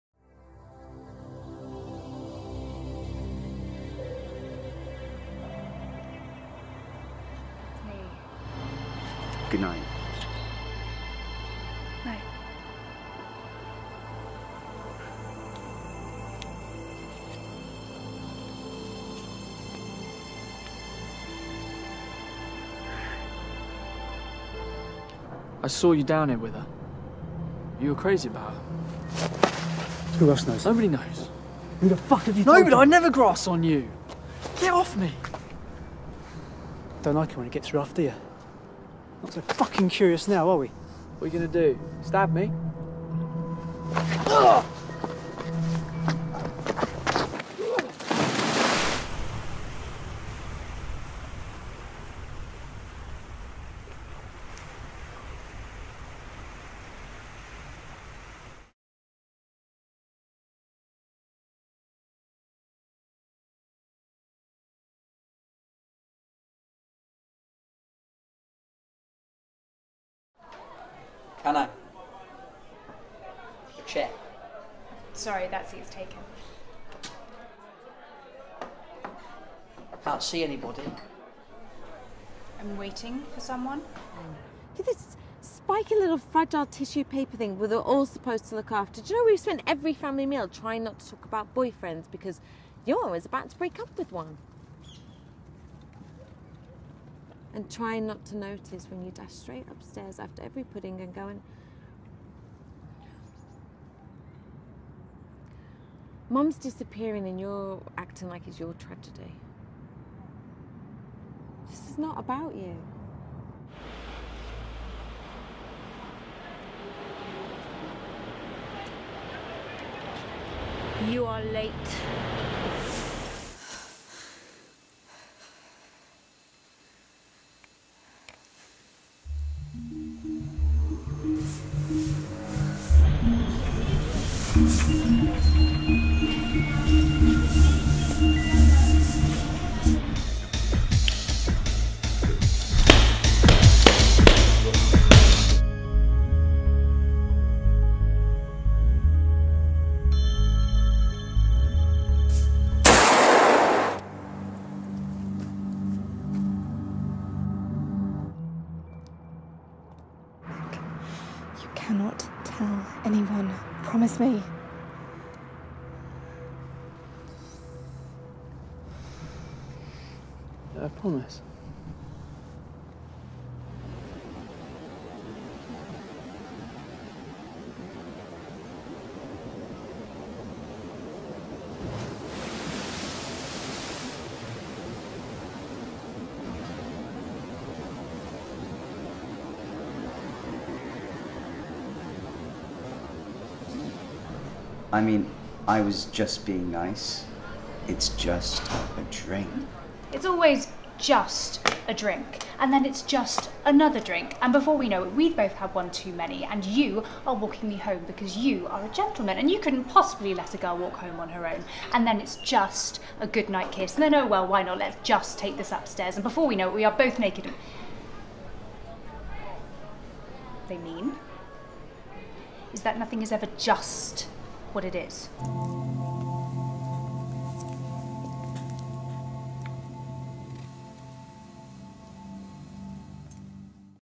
Laura Marie Linck - Directing Showreel
French/English Director based in London